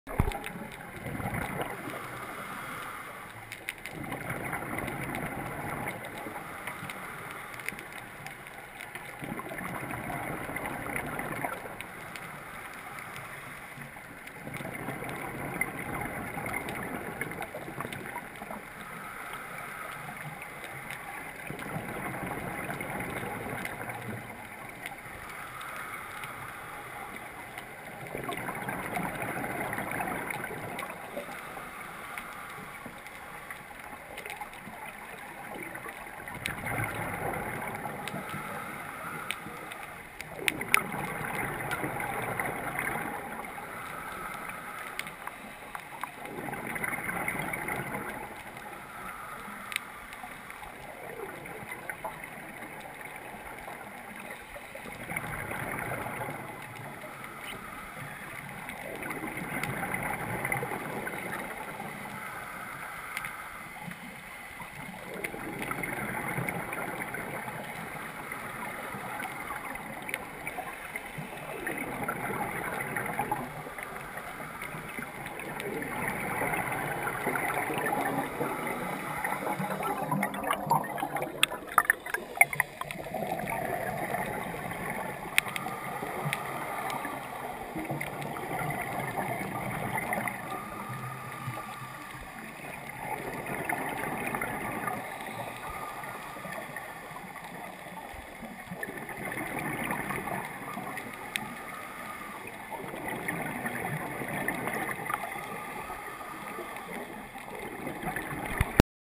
Turtle Shot
Shows the distance from the turtle and camera technique (hand held) to get the Turtle shot